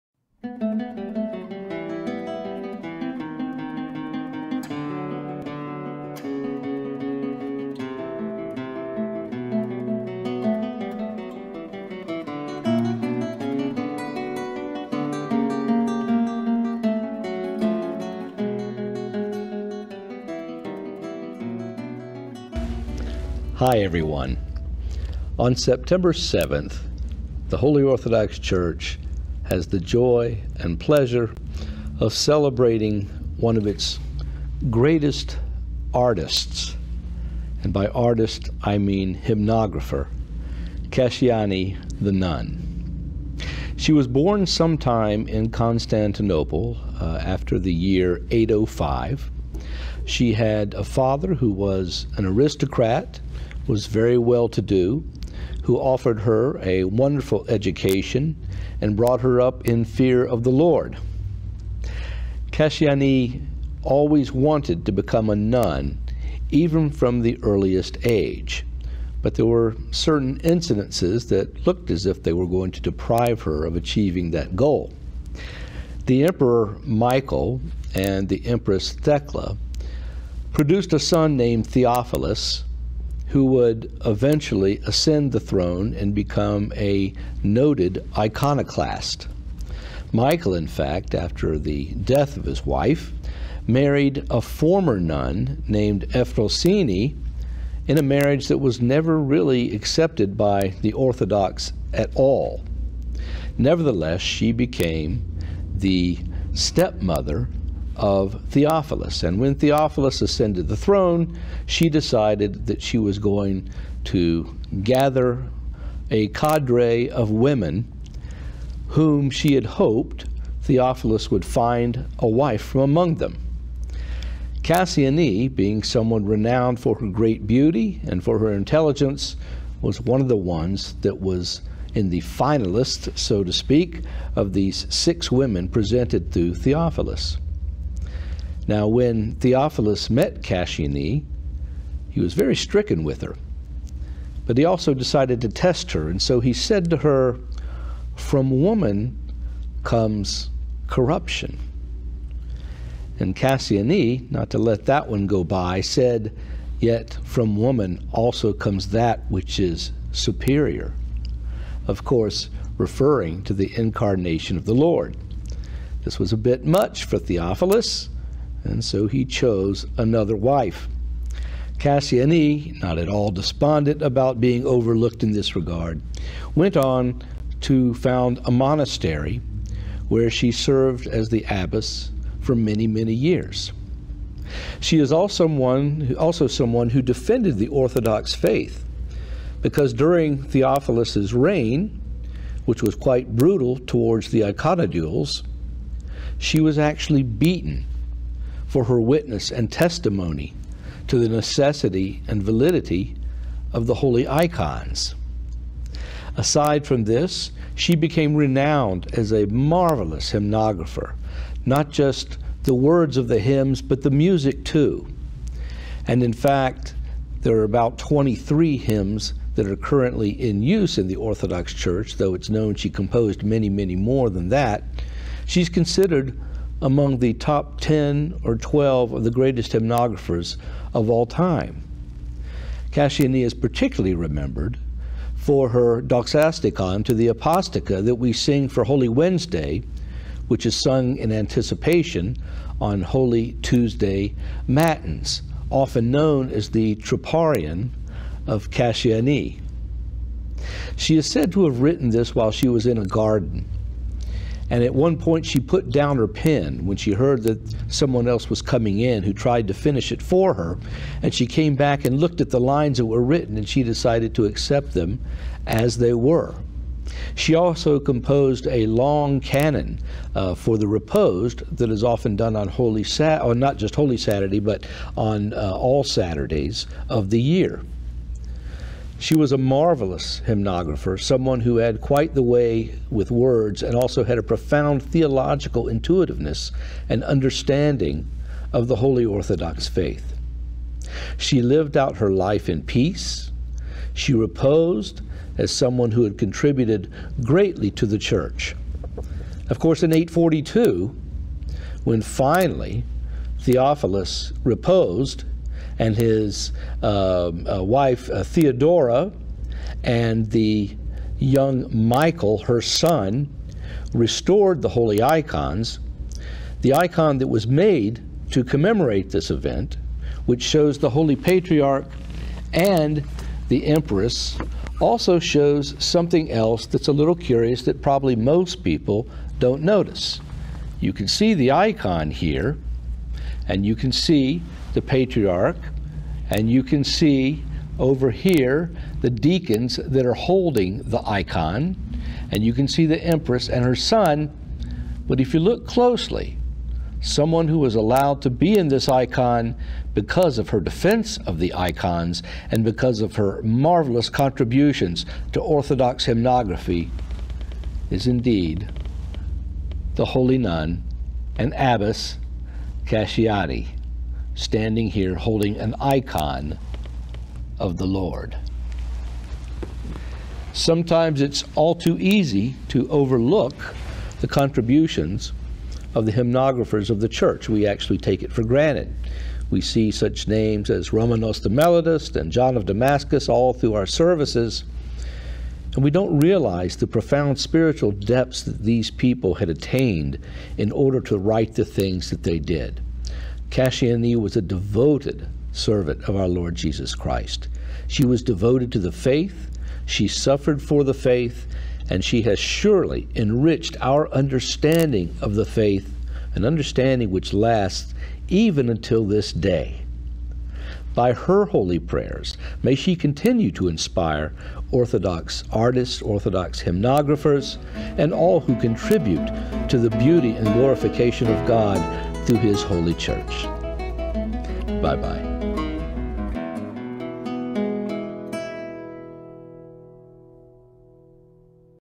Hidden Saints - Kassiane the Hymnographer (sep 7)
This weekly series features the lesser known saints of the Orthodox Church, narrated by Fr. Steven Ritter, Pastor of St. James Orthodox Church in Buford, Georgia